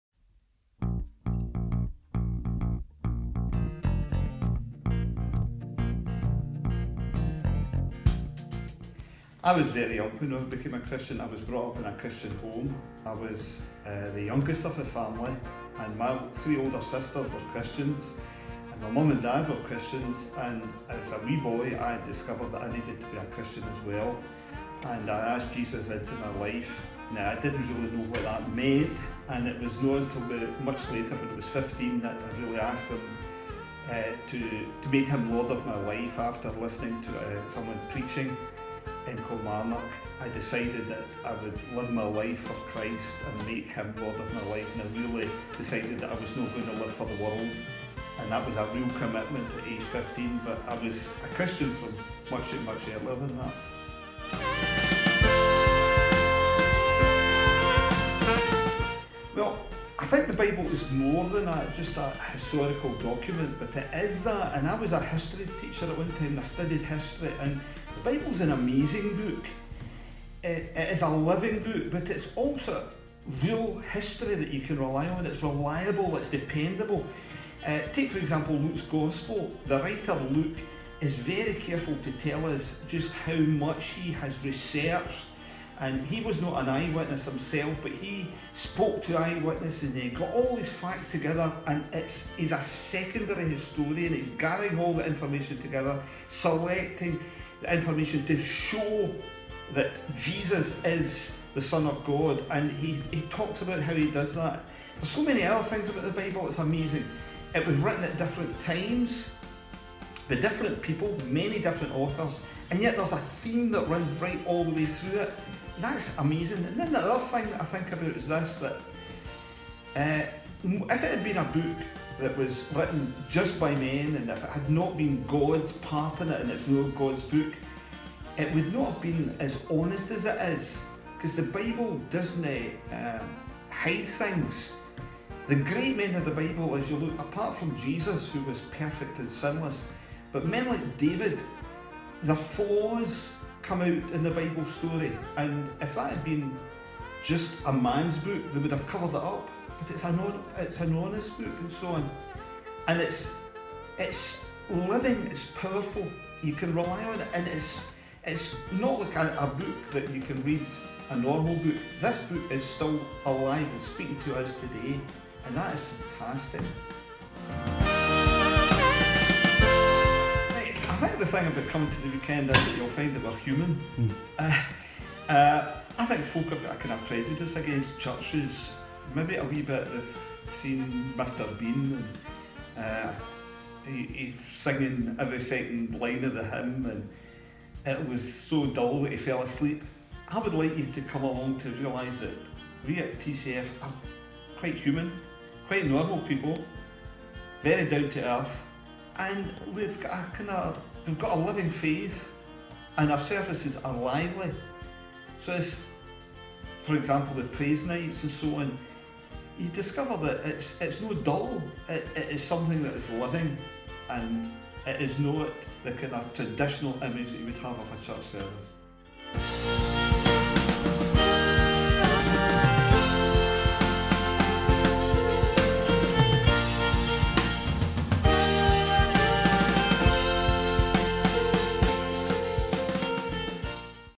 Easter Testimony 3
In this video Robert tells us about the amazing things he sees in the Bible.